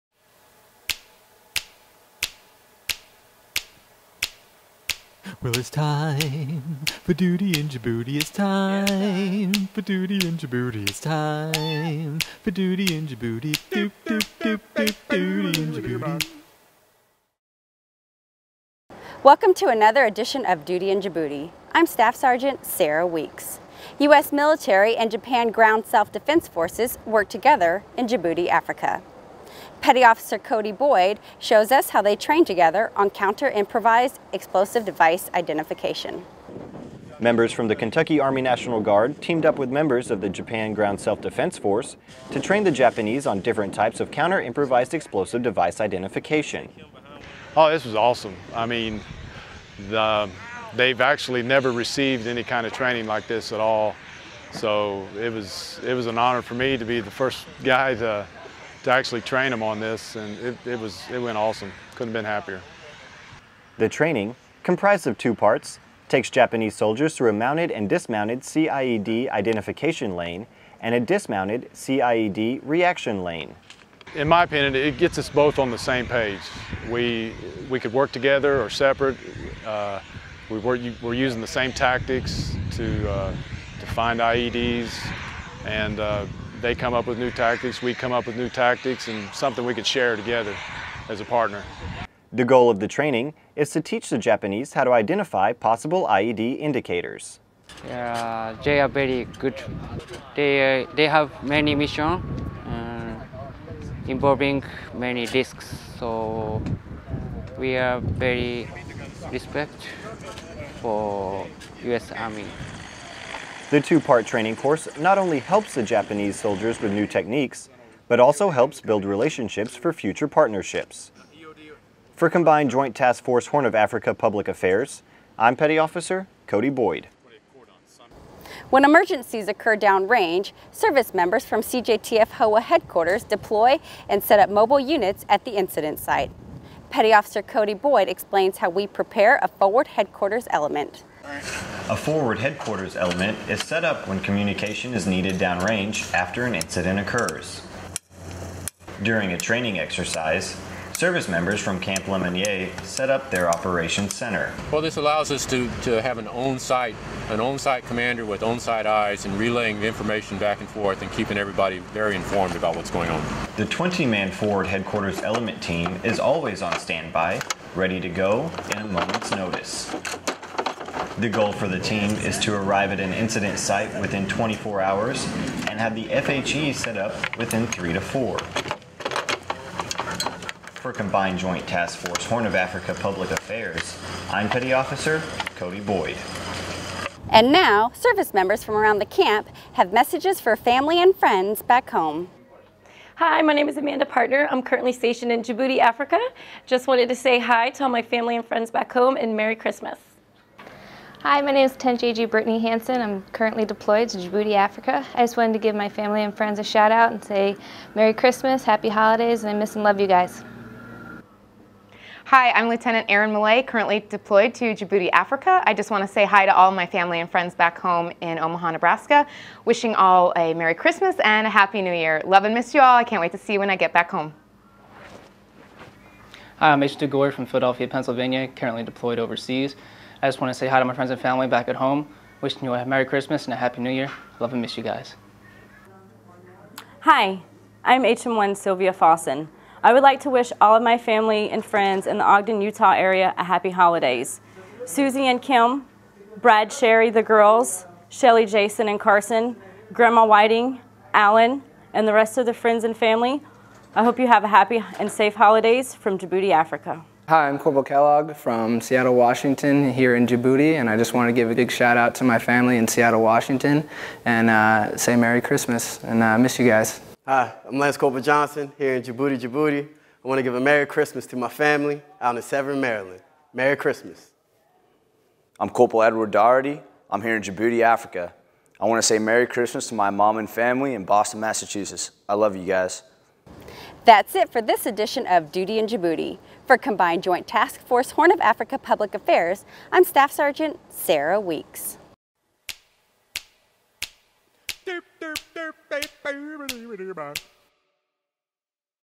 Duty in Djibouti Dec. 14, 2012
Welcome to another edition of duty in Djibouti.  I'm SSgt Sara Weeks.  U.S. Military and Japan Ground Self-Defense Force members work together as they train on Counter-Improvised Explosive Device Identification. MC2 Cody Boyd also shows up how CJTF-HOA sets up a Forward Headquarters Element.  And, as always we have the weekly Shoutouts!  Also available in High Definition.